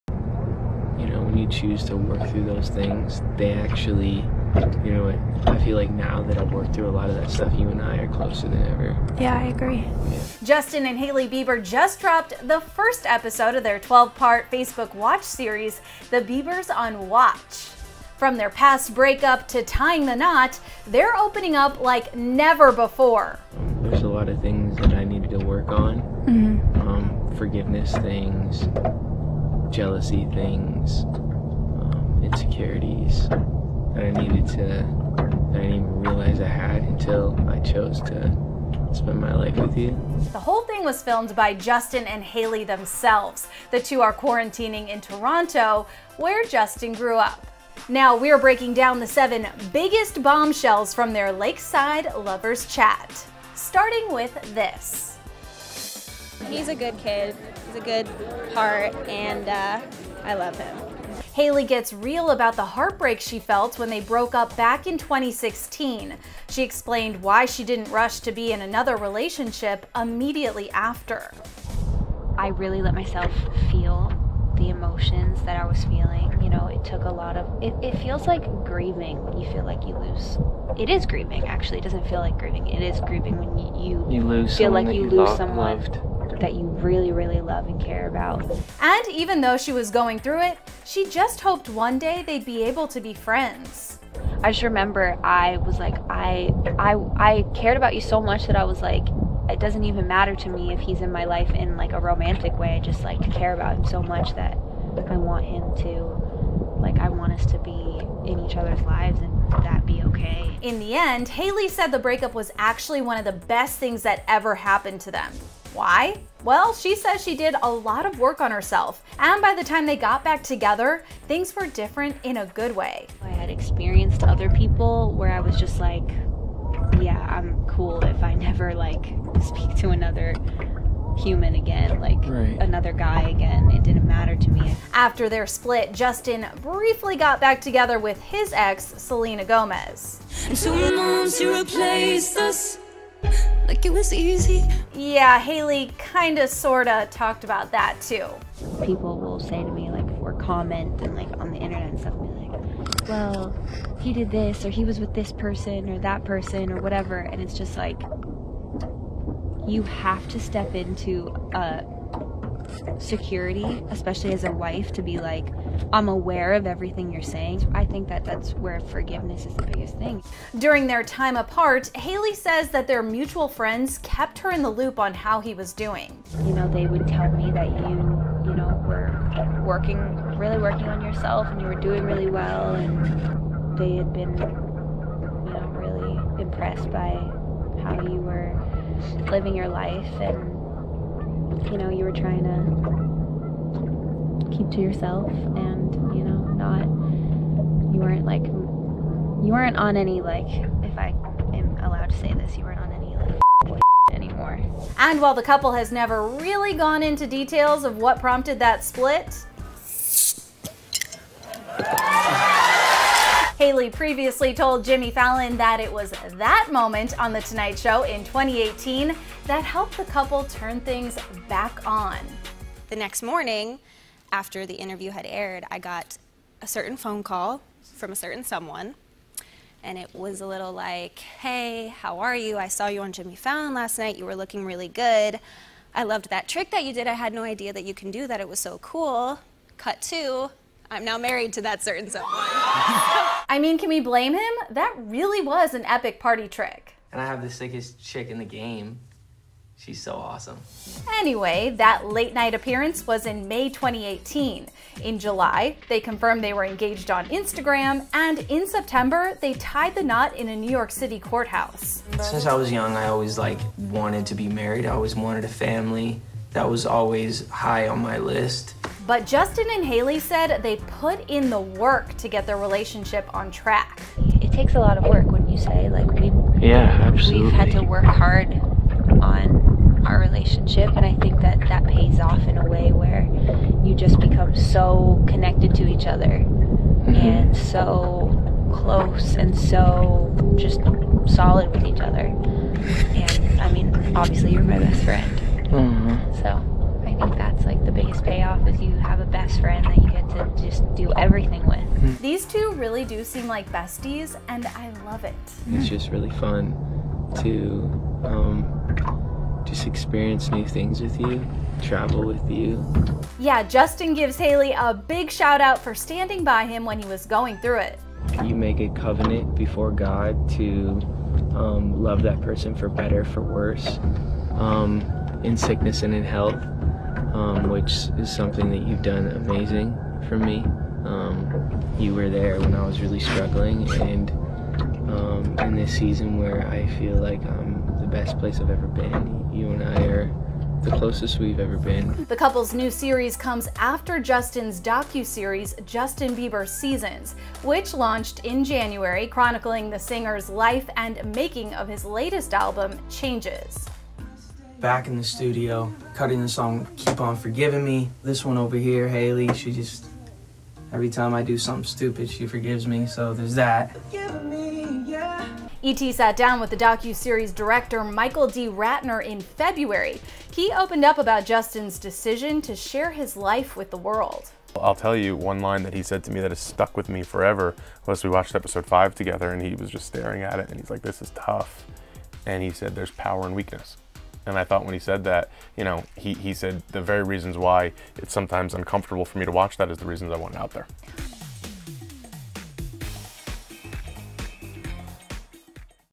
Justin and Hailey Bieber Open Up Like NEVER BEFORE About Their 2016 Breakup
Justin and Hailey get candid about their 2016 split in their new Facebook Watch series, ‘The Biebers on Watch.’